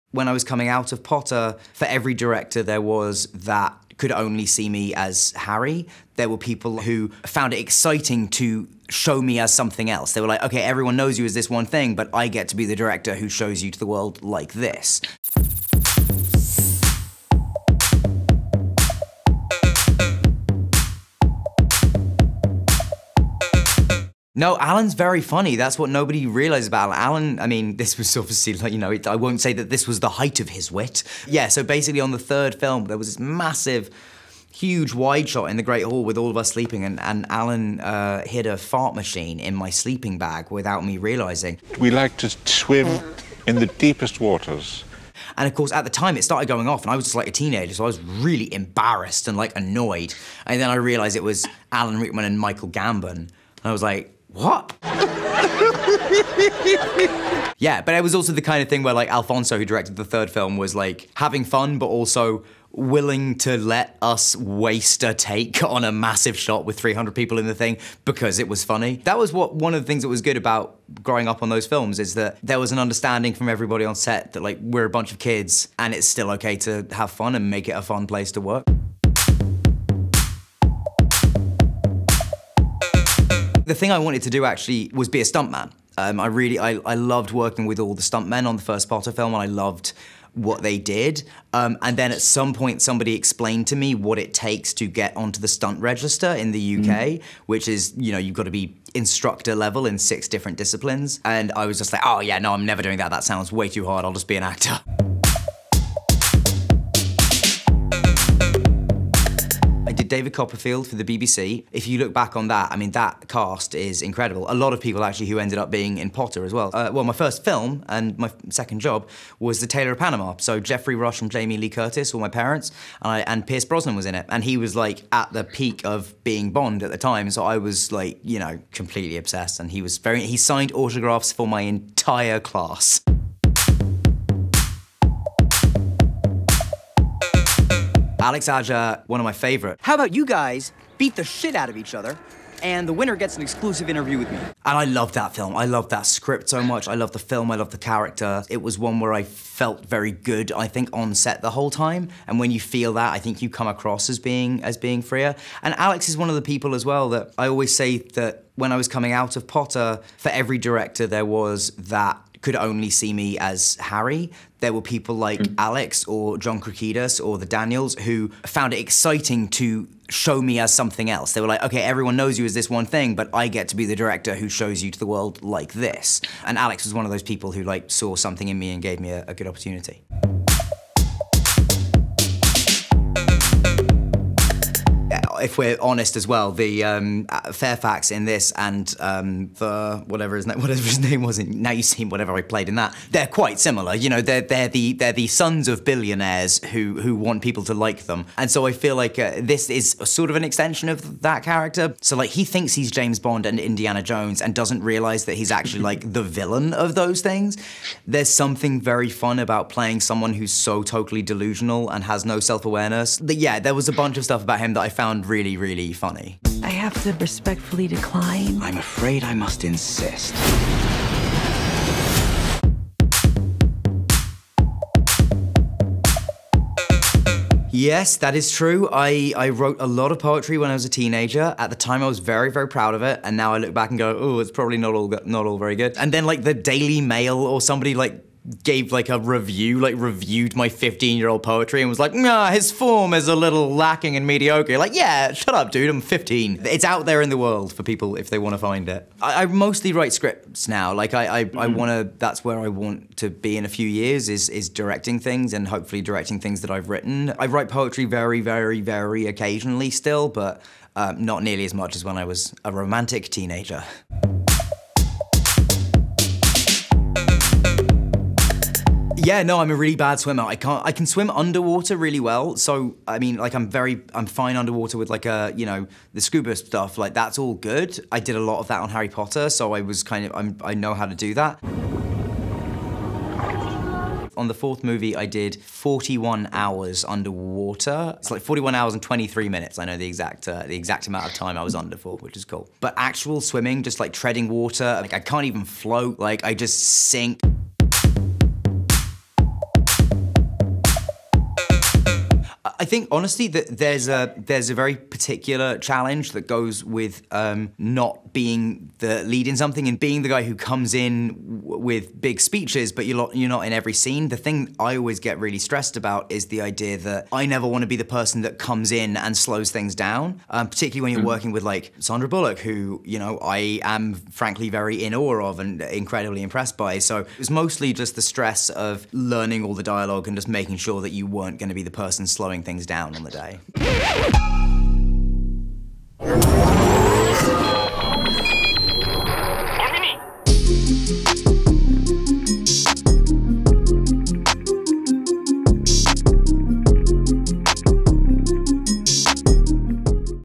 Daniel Radcliffe : le jour où Rogue l'a pranké avec une machine à pets pendant Harry Potter 😭
Un jour, le professeur Rogue l'a pranké avec une machine a pets sur le tournage d'Harry Potter 😭

Il est actuellement à l'affiche du film "Le Secret de la cité perdue", qui sort aujourd'hui en salles, et pour l'occasion, Daniel Radcliffe nous révèle ce qu'on ne savait pas sur lui !